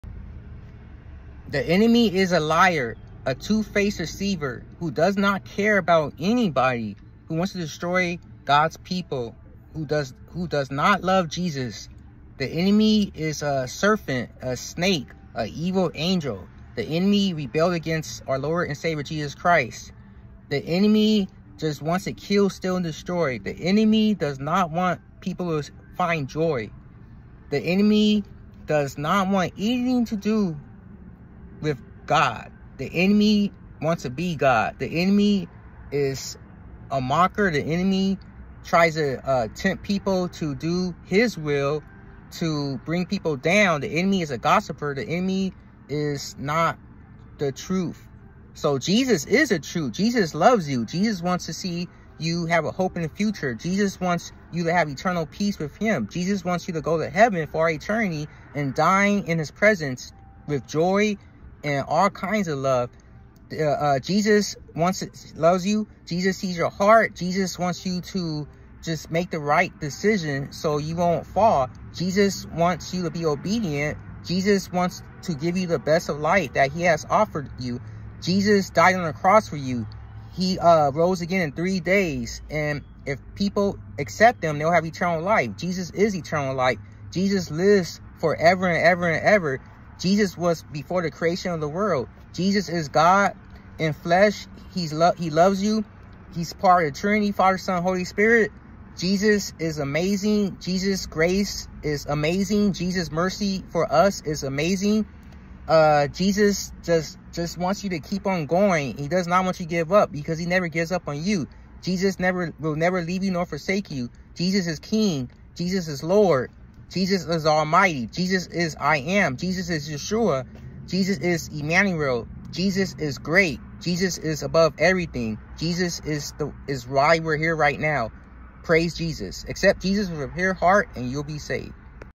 Jesus vs the devil. Jesus, Jesus, Jesus, our King of kings and Lord of lords🙏❤️ Accept Jesus🙏❤️
The devil wants to destroy everyone. Jesus wants to save everyone🙏❤️

 Jesus Is God🙏❤️

Accept Jesus with a pure❤️and you will be saved🙏❤️